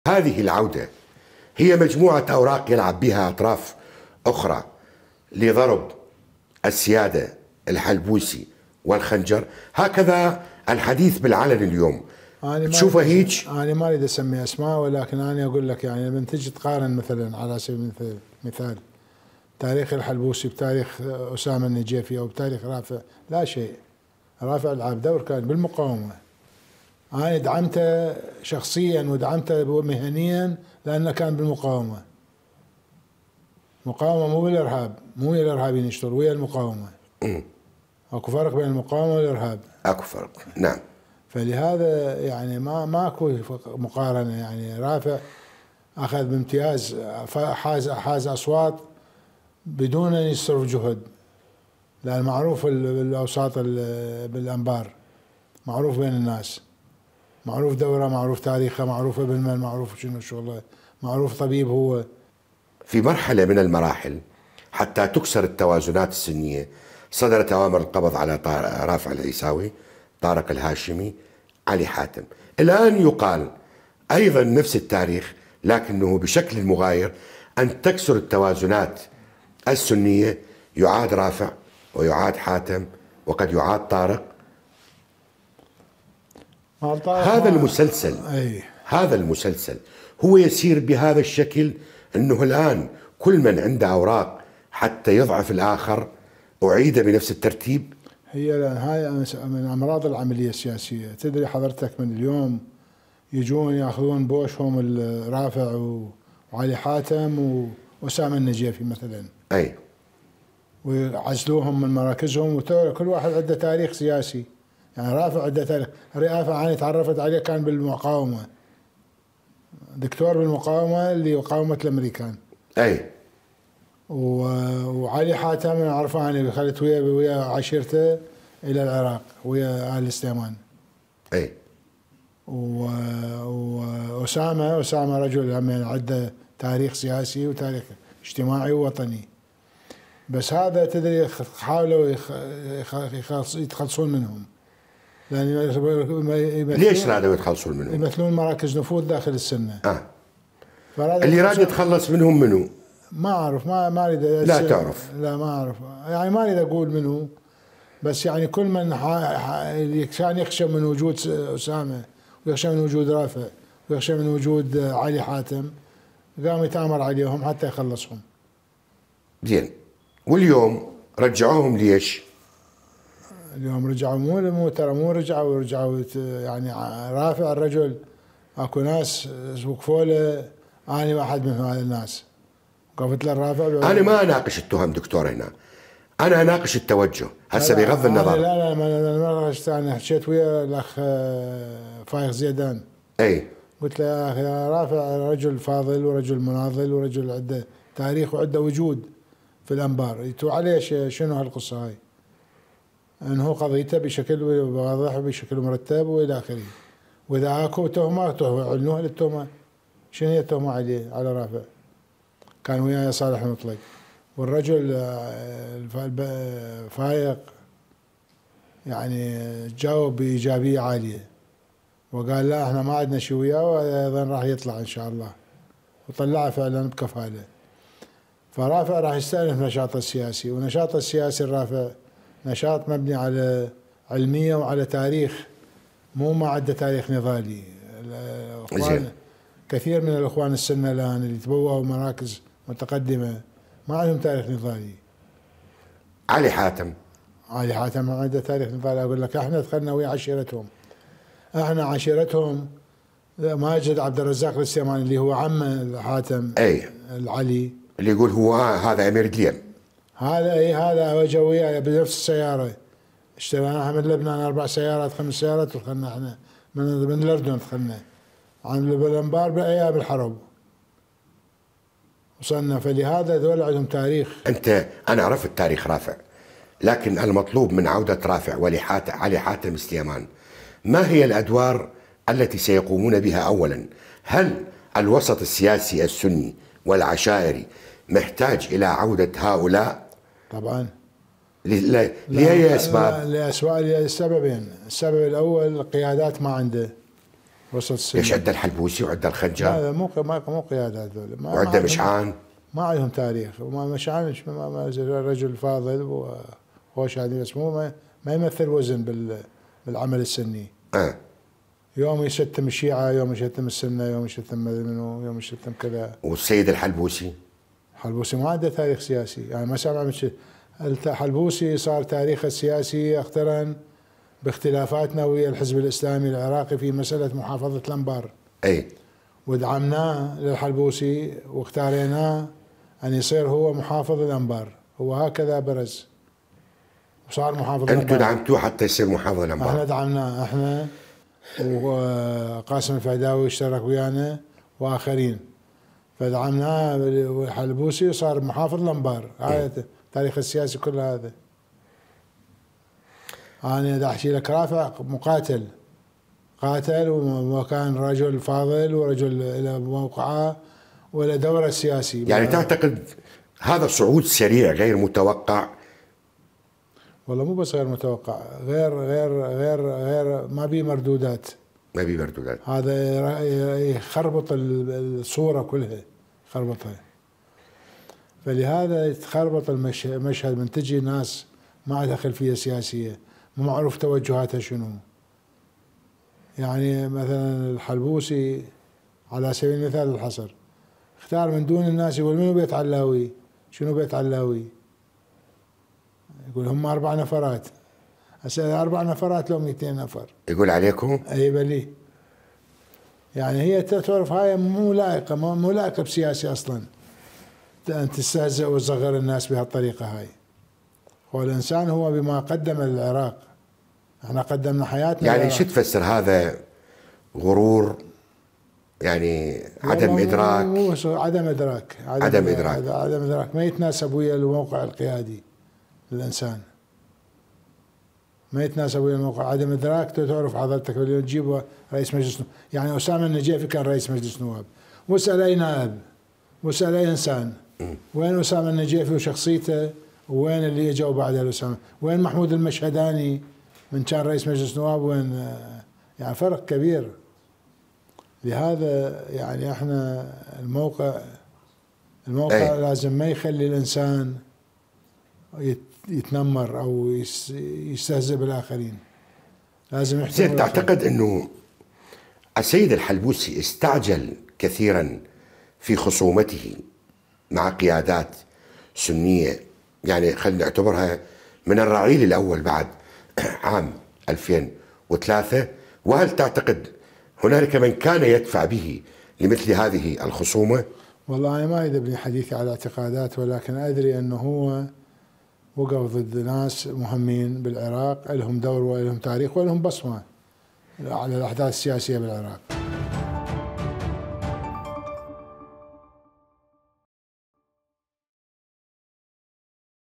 اياد علاوي يكشف معلومات خطيرة عن محمد الحلبوسي وقيادات السنة
خميس الخنجر و رافع العيساوي و علي حاتم سليمان و اسامة النجيفي  و مشعان الجبوري
موقع قناة التغيير الفضائية الالكتروني